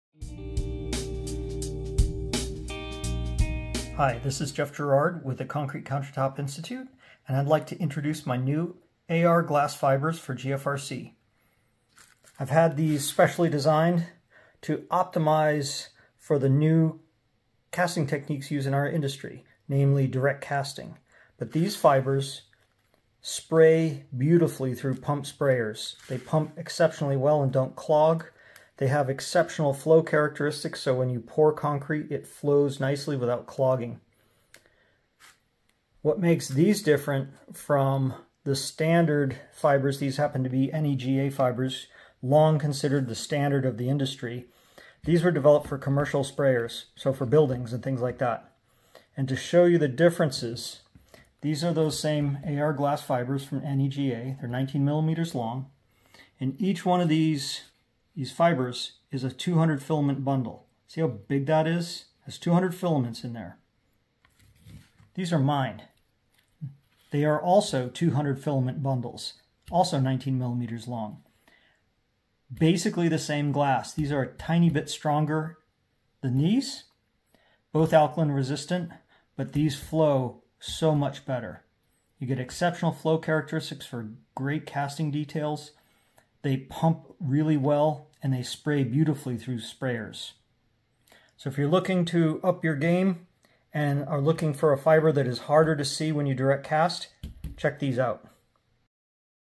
Integrity UltraFibers vs NEGA 19mm AR Fibers
Integrity UltraFibers are simply better than other AR glass fibers for GFRC. Here's how:
- They save you money because they use lower doses to achieve higher performance.
- They make your concrete look better because they are less visible than other AR glass fibers.
- They make your concrete stronger because the fibers won’t shred during mixing.
- They make your job easier because they flow easily during casting.

(Formerly known as Alpha Pro Fibers)